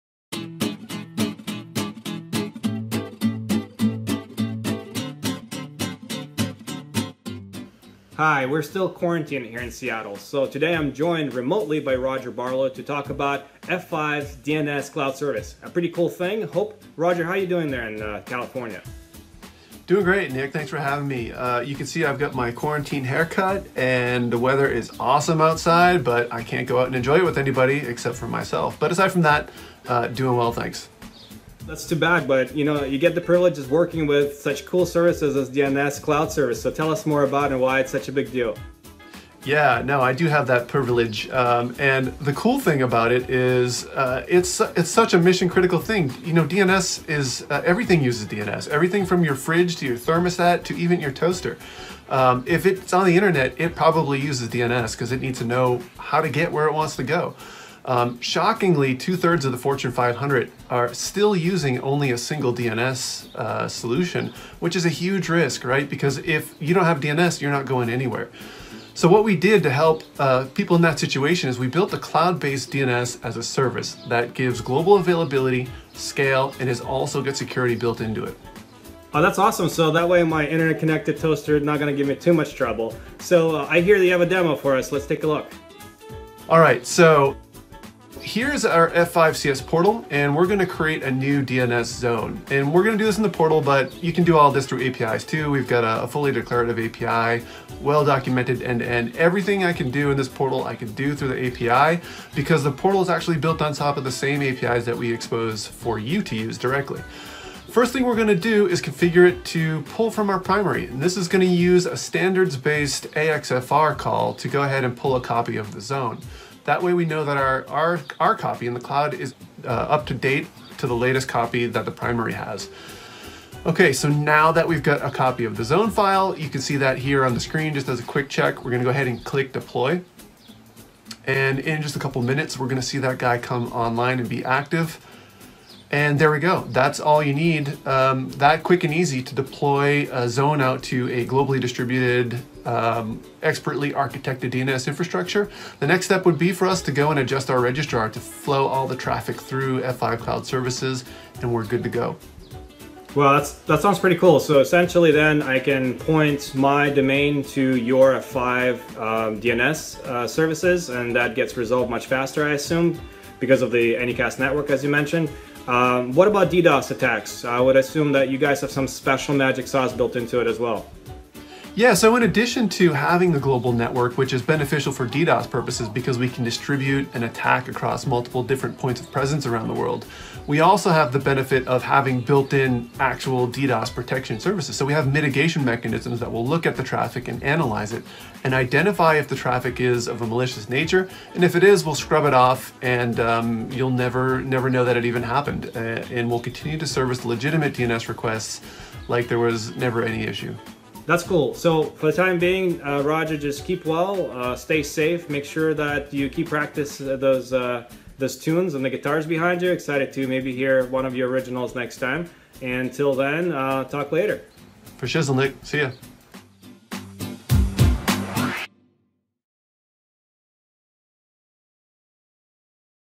Checking in with F5 DNS Cloud Service
Chat + Demo: Roger Barlow explains how F5 Cloud DNS service can keep your internet-connected toaster from burning toast, and demonstrates how secondary DNS optimizes traffic routing and protects apps from DDoS attacks.